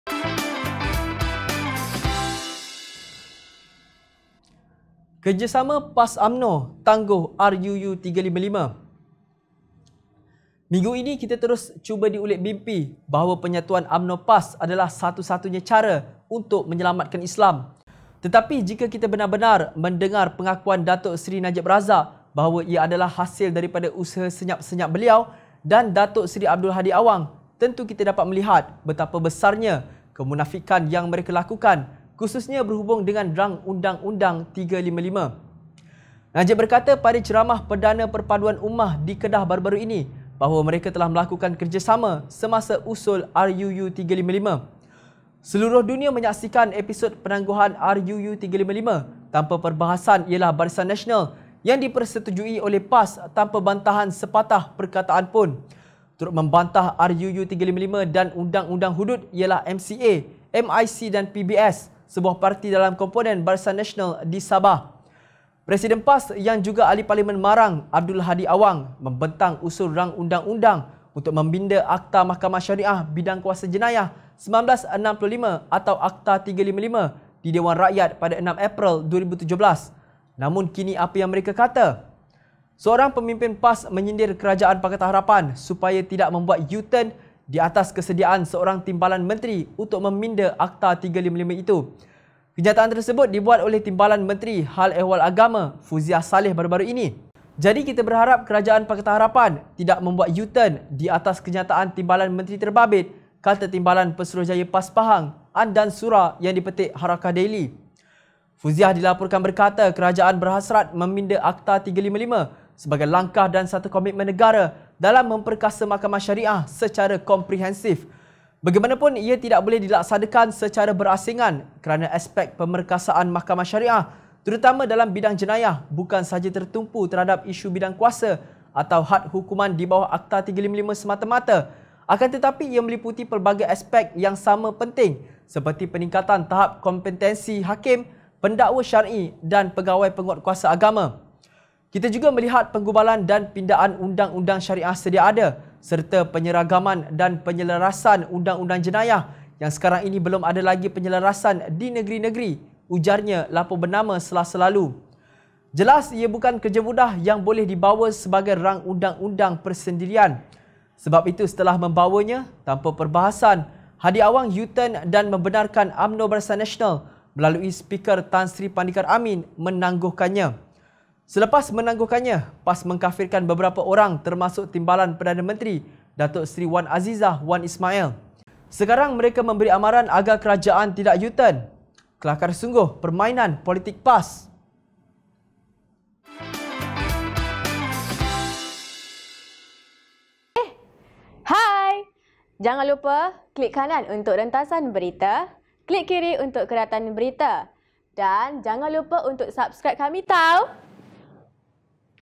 KERJASAMA PAS-UMNO TANGGUH RUU 355
Selepas menangguhkannya, PAS mengkafirkan beberapa orang termasuk Timbalan Perdana Menteri, Datuk Seri Wan Azizah Wan Ismail.

Sekarang mereka memberi amaran agar Kerajaan tidak U-TURN.
Kelakar sungguh permainan politik PAS